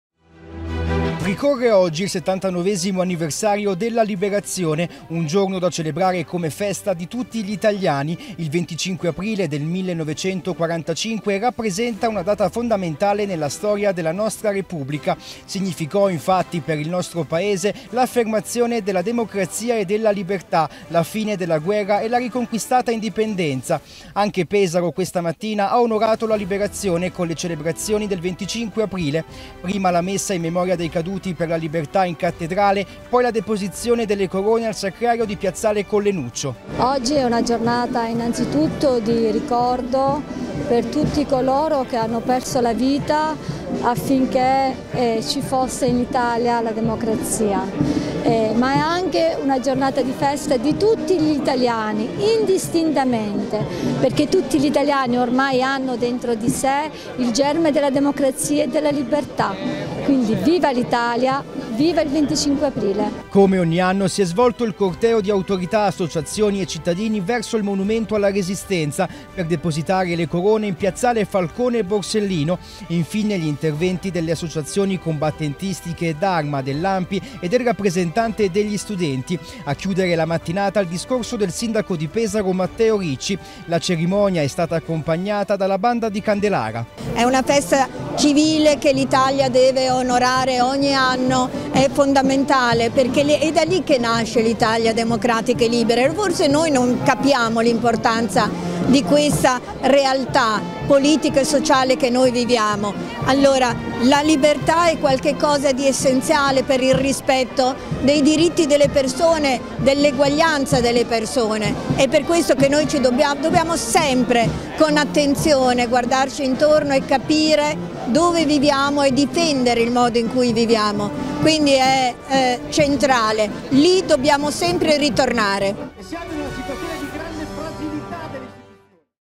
Festa della Liberazione: le celebrazioni a Pesaro
A Pesaro si sono svolte le celebrazioni in occasione del 25 Aprile con la messa in Cattedrale e il corteo verso il Monumento alla Resistenza.